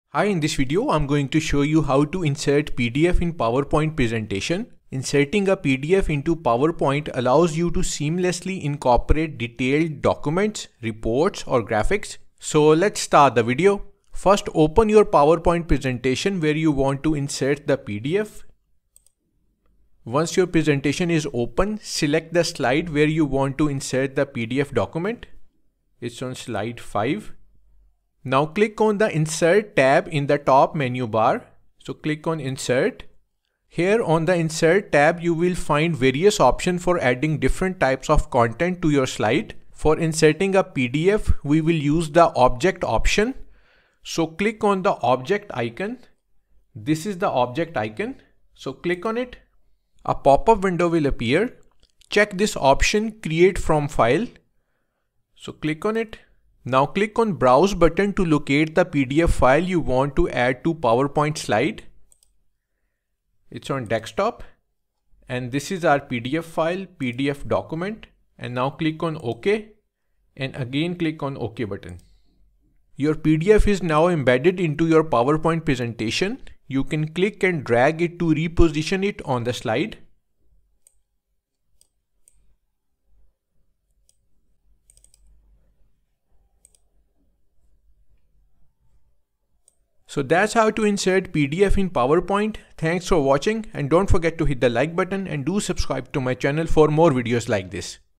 How to Insert PDF in PowerPoint
This video guides about how to insert pdf in powerpoint presentation.

Adding a PDF into PowerPoint allows you to seamlessly incorporate detailed documents, reports, or graphics, enhancing the visual and informational depth of your presentation.

To learn how to insert pdf in powerpoint, simply follow the step-by-step guide.

Here are the Steps to Insert PDF into PowerPoint:

1. Open PowerPoint presentation and select the slide where you want to add the PDF.

2. Click on the "Insert" tab in the top menu bar.

3. Click on the "Object" icon.

4. In pop-up window check this option "create from file".

5. Click on the browse button to locate the pdf file you want to add to powerpoint slide and click "OK".

PDF file will now be embedded into your PowerPoint presentation.

So that's how to insert pdf in powerpoint. If you encounter any issue while adding PDF in PPT slides, ask me in the comments section below and i will surely help.